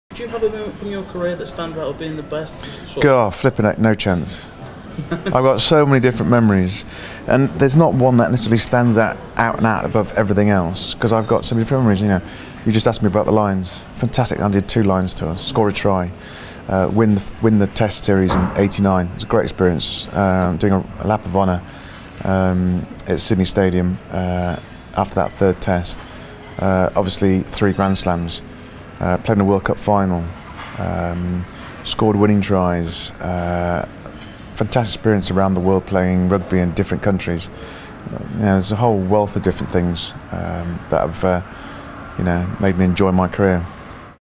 Rory Underwood interview
Chris Simmons caught up with England and British and Irish Lions legend Rory Underwood as he flew in to London today to discuss how to play the "wing" position in Rugby Union. Over a cup of coffee and a muffin at the Hilton hotel just outside Heathrow's Terminal 4, Chris asked Rory about his career highlights. If you'd like to see the full version of the interview, see it